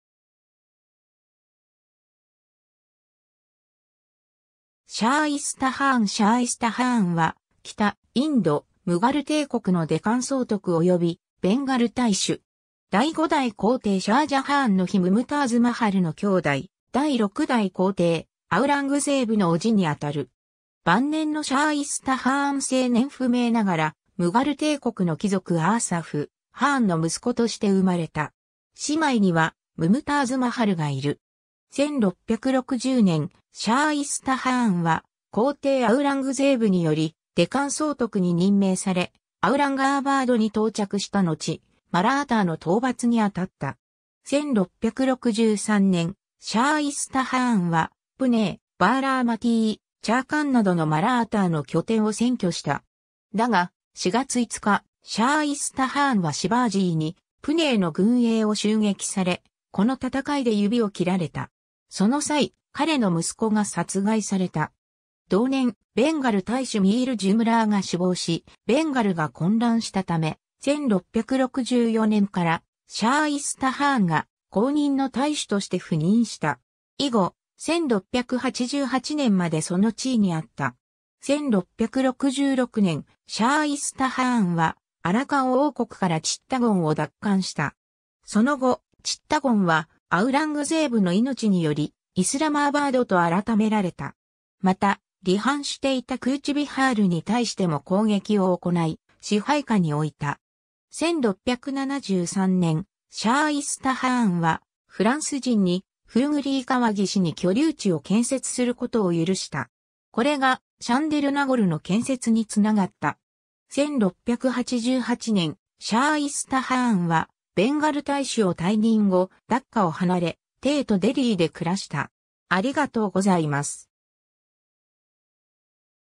シャーイスタ・ハーン
シャーイスタ・ハーン シャーイスタ・ハーン（Shaista Khan, 生年不詳 - 1694年）は、北インド、ムガル帝国のデカン総督及びベンガル太守。
第5代皇帝シャー・ジャハーンの妃ムムターズ・マハルの兄弟、第6代皇帝アウラングゼーブの叔父にあたる。
 晩年のシャーイスタ・ハーン 生年不明ながら、ムガル帝国の貴族アーサフ・ハーンの息子として生まれた。
姉妹にはムムターズ・マハルがいる。
 1660年、シャーイスタ・ハーンは皇帝アウラングゼーブによりデカン総督に任命され、アウランガーバードに到着したのち、マラーターの討伐にあたった。
 1663年、シャーイスタ・ハーンはプネー、バーラーマティー、チャーカンなどのマラーターの拠点を占拠した。
だが、4月5日、シャーイスタ・ハーンはシヴァージーにプネーの軍営を襲撃され、この戦いで指を切られた。
その際、彼の息子が殺害された。
 同年、ベンガル太守ミール・ジュムラーが死亡し、ベンガルが混乱したため、1664年からシャーイスタ・ハーンが後任の太守として赴任した。
以後、1688年までその地位にあった。
 1666年、シャーイスタ・ハーンはアラカン王国からチッタゴンを奪還した。
その後、チッタゴンはアウラングゼーブの命により、イスラマーバードと改められた。
また、離反していたクーチ・ビハールに対しても攻撃を行い、支配下に置いた。
 1673年、シャーイスタ・ハーンはフランス人にフーグリー川岸に居留地を建設することを許した。
これがシャンデルナゴルの建設に繋がった。
 1688年、シャーイスタ・ハーンはベンガル太守を退任後、ダッカを離れ、帝都デリーで暮らした。